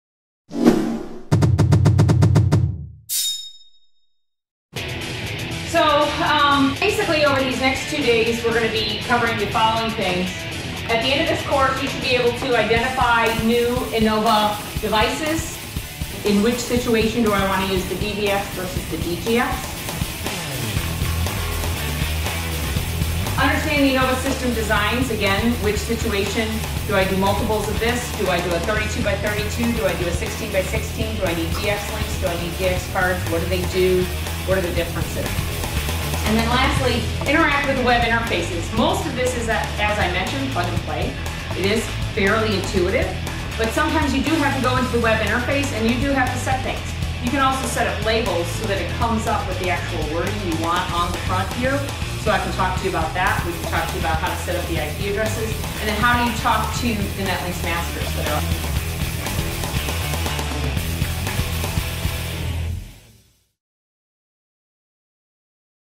Enova Solutions Design & Implementation Trailer
This is a trailer for the Enova Solutions Design and Implementation course. This two-day class will teach you everything you need to know to design, install, and configure an Enova DVX-215X/315X Series Presentation Switcher or Enova DGX 16/32 digital media switcher + control.

If you complete the course and associated exams, you will receive the Enova Solutions Certified training certificate.